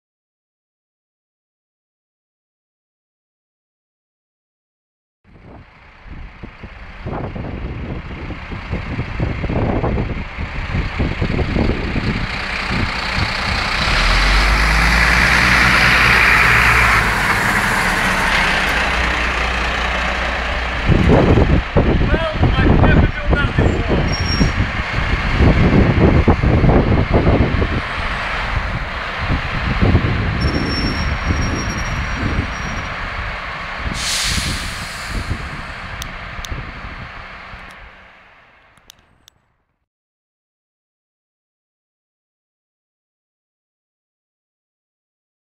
Curly, Larry and Maureen movie maker.wmv
Watch Manager Rob Turner escorts Curly, Larry & Maureen out of the Engine House at Runcorn Fire Station. just in time!